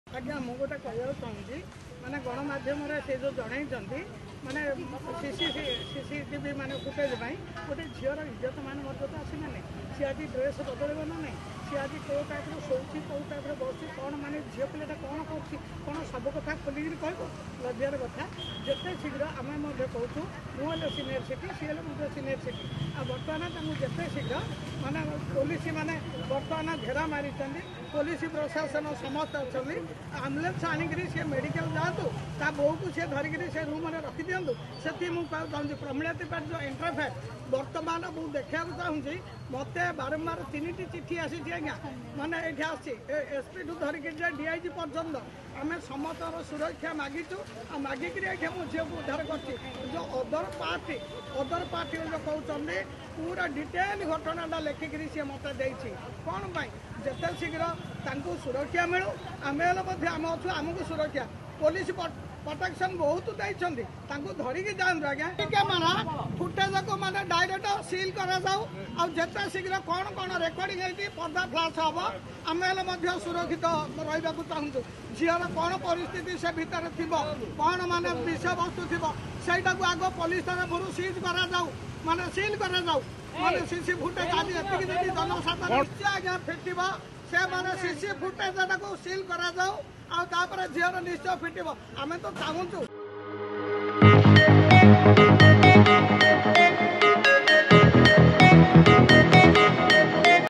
‘ସିସିଟିଭି ଫୁଟେଜକୁ ସିଲ୍‌ କର’ || Tapaswini-Sumit Marriage Case Update || Berhampur || Odisha Reporter
''ଗୋଟେ ଝିଅର କଣ କିଛି ଇଜତ ନାହିଁ । ସିସିଟିଭି ଲାଗିଥିଲେ ସେ ସବୁ ଦେଖିବେ'' । ଶୁଣନ୍ତୁ ଏନେଇ କଣ କହୁଛନ୍ତି ଡିମ୍ପଲଙ୍କ ସହ ବସିଥିବା ସମାଜ ସେବି ।